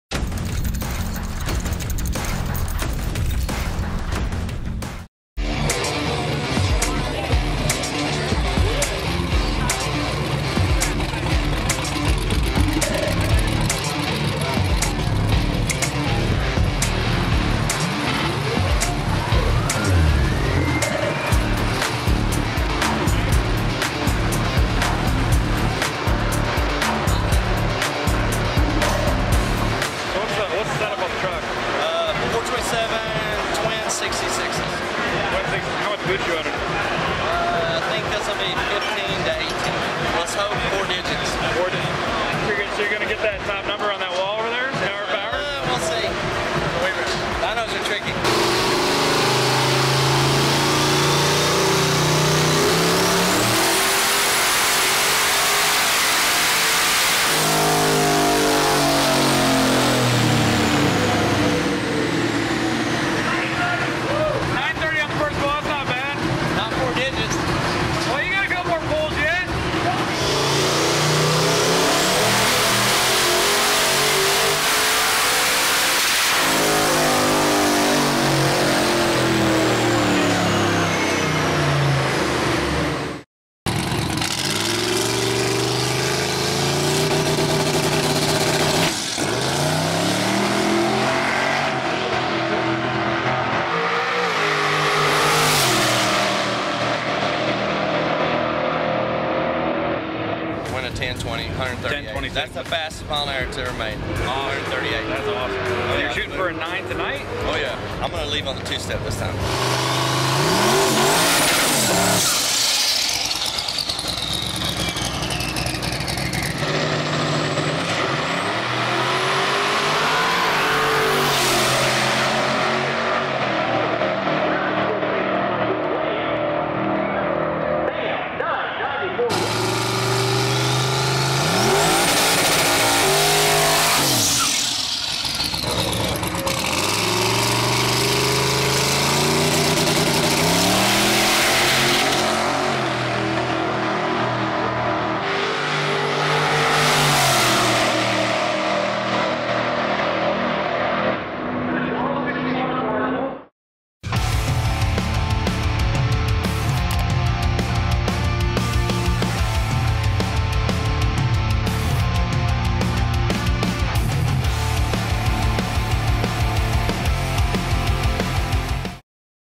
TWIN TURBO Silverado - Single Cab MONSTER!
JUST shy of 1000whp - This badass Silverado rang in at 930whp the night before laying down a few 10 second passes!  Our friends over at UltraBlue Traction Compound brought their shop truck out to Street Car Takeover - Phoenix, and decided to have a little fun with it!  While still getting it dialed in they made a few passes attempting to get into the 9’s.  While they didn’t hit that goal at this event, they aren’t giving up.  We have our eyes on, waiting to see what this truck to see what it’s REALLY capable of!